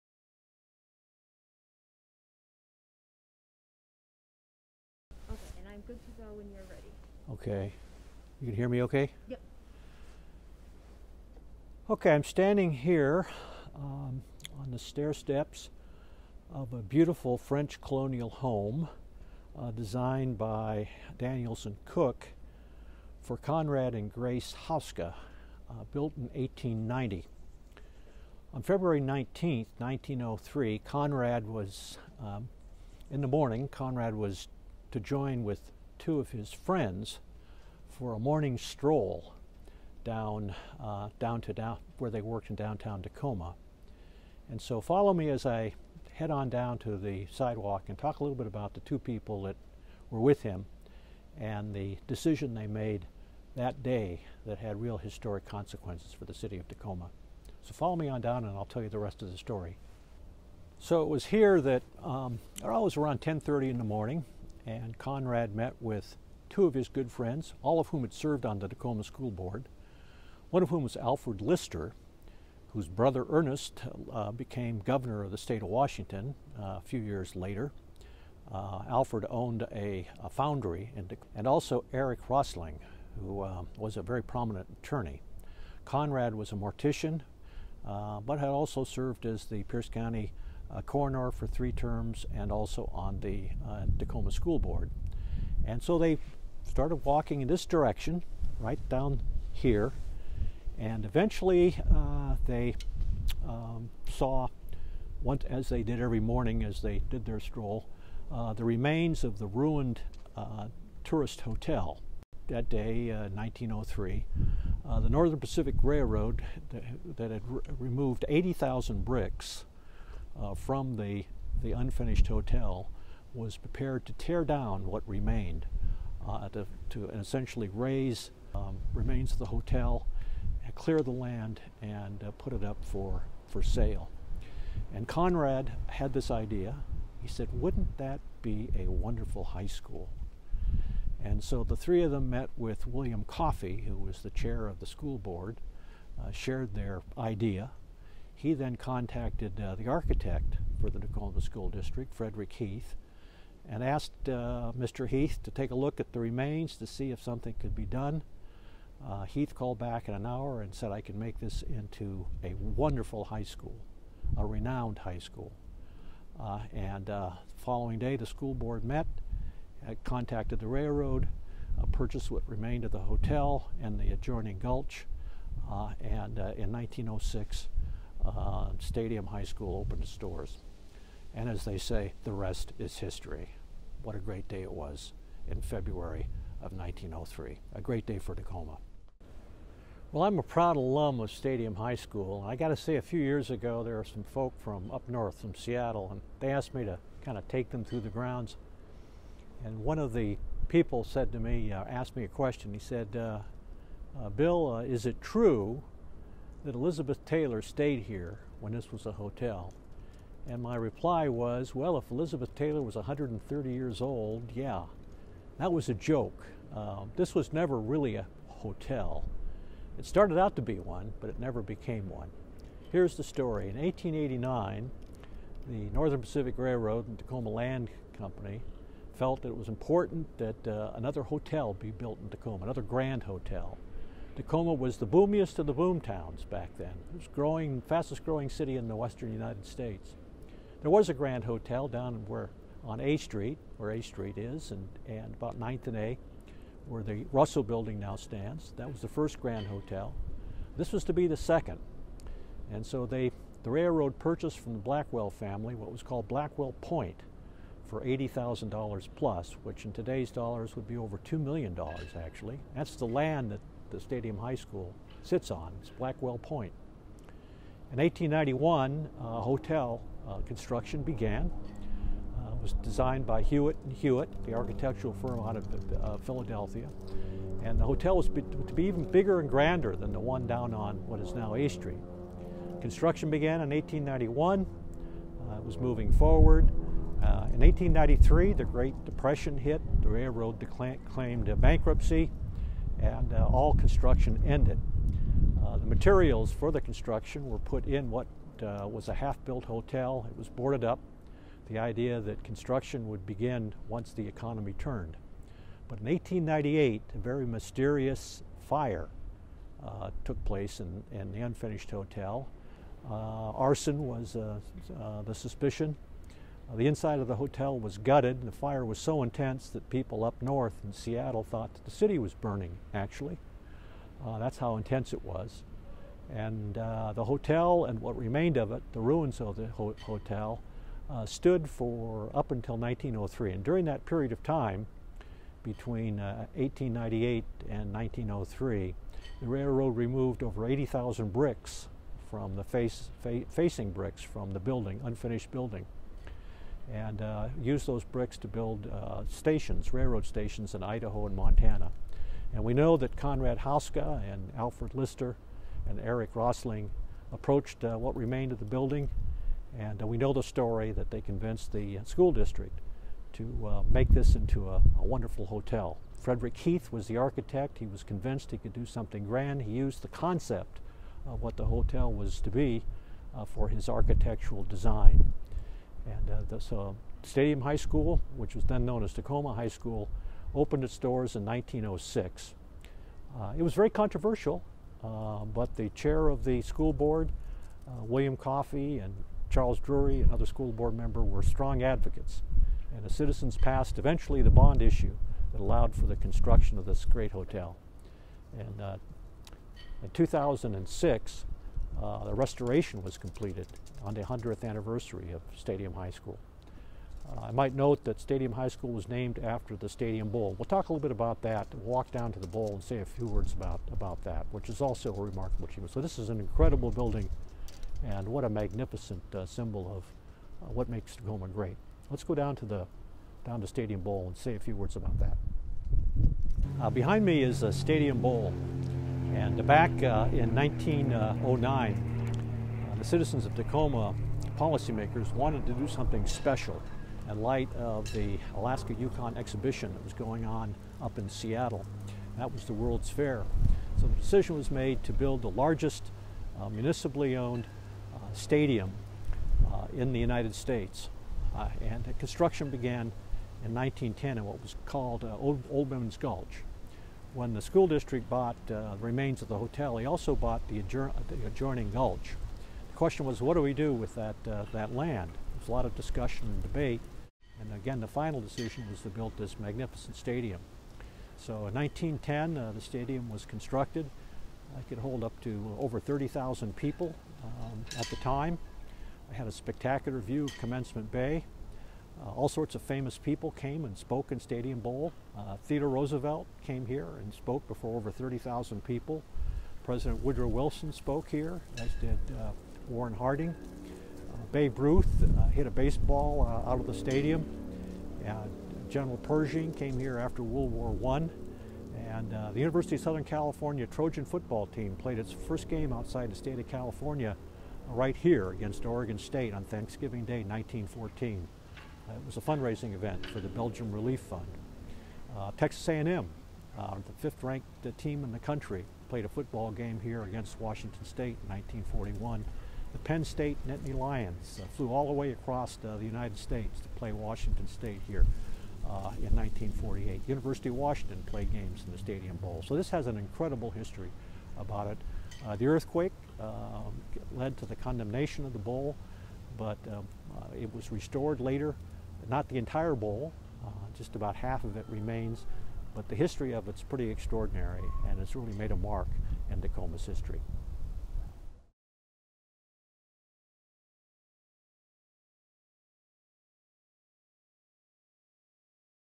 Historic Homes Tour (2020): Stadium High School & Conrad Hoska
This video explores the connection between the historic Hoska House and Stadium High School. Dr. Bill Baarsma, President of Tacoma Historical Society, examines the genesis of the idea to create a high school from the burned down rubble of a planned hotel.

Production of this video was produced as part of a walking tour of the historic Stadium District by the Tacoma Historical Society, with support from the Pierce County Landmarks and Historic Preservation Commission and Tacoma Creates. You can view the full tour of the Stadium Historic District